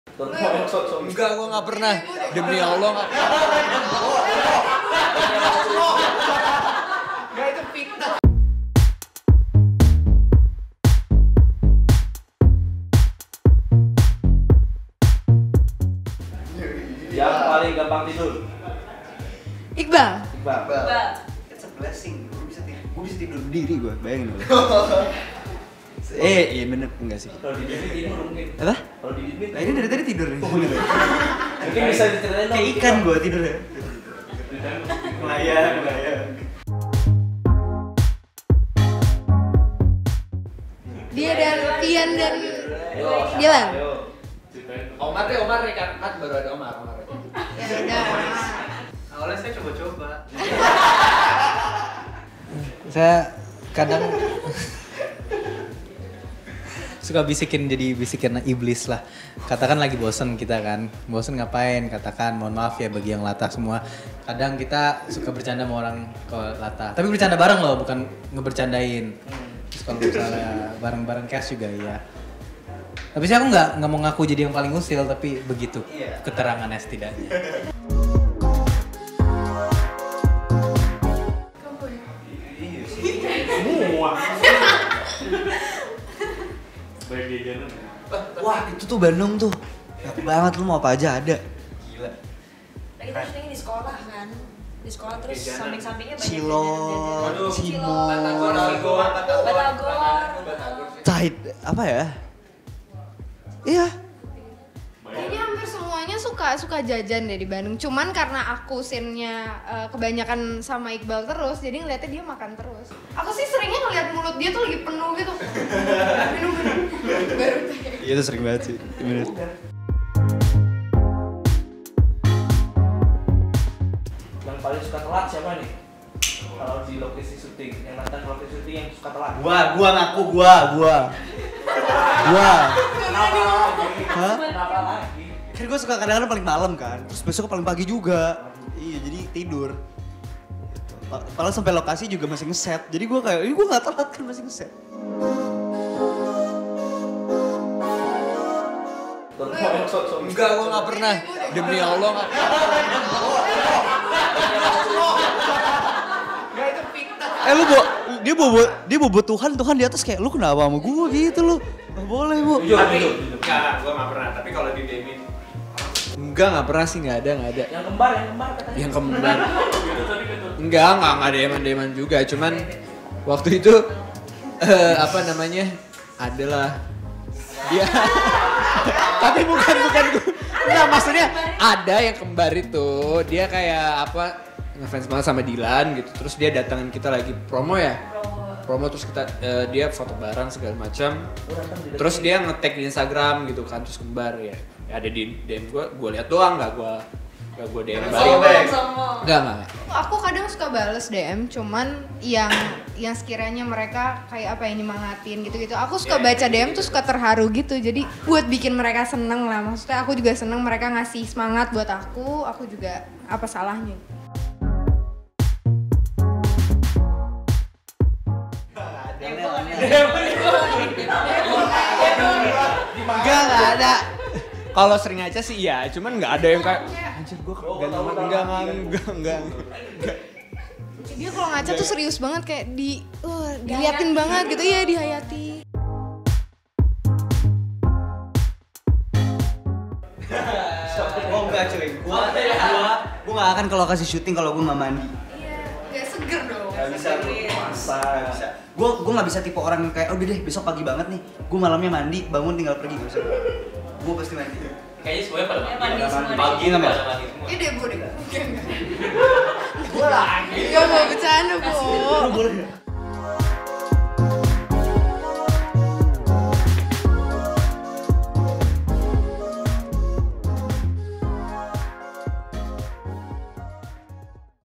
Iqbaal Ramadhan, Vanesha Prescilla & Cast MILEA: SUARA DARI DILAN Main Saling Tunjuk
KAPANLAGI.COM - Iqbaal Ramadhan, Vanesha Prescilla, dan cast MILEA: SUARA DARI DILAN kami ajak untuk main saling tunjuk. Iqbaal dan Debo jadi 2 orang yang paling sering ditunjuk. Penasaran? Tonton videonya!